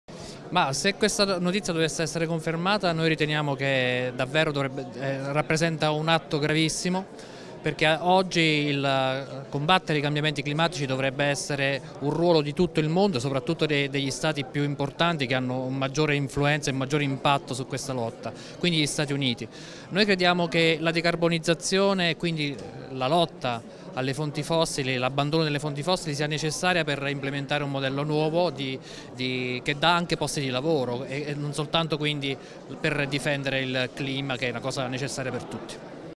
Clima, Pedicini (M5S): "Decisione di Trump di abbandonare accordi Parigi sarebbe gravissima"
(Agenzia Vista) Bruxelles, 31 maggio 2017
Secondo il sito Axios la Casa Bianca avrebbe deciso di ritirare la firma dal patto sul cambiamento climatico. Il tweet del presidente Usa Donadl Trump: "La mia decisione nei prossimi giorni". / Twitter